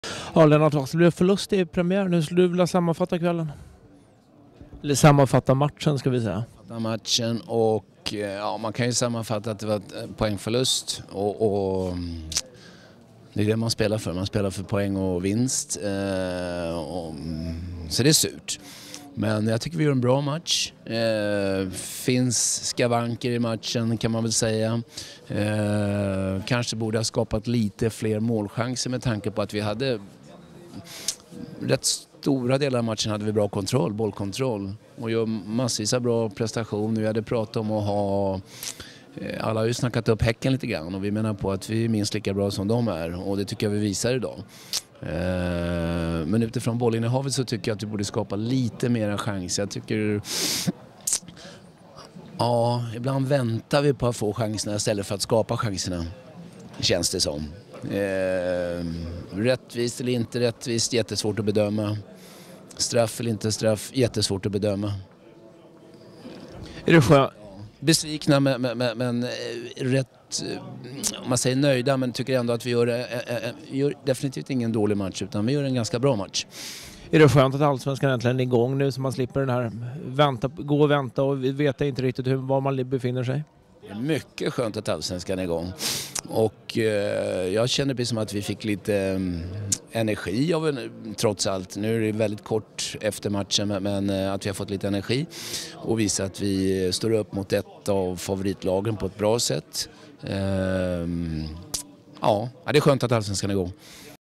Lennart Wass efter BK Häcken - DIF
Djurgården förlorade i Borås, och här analyserar tränaren Lennart Wass den allsvenska premiären.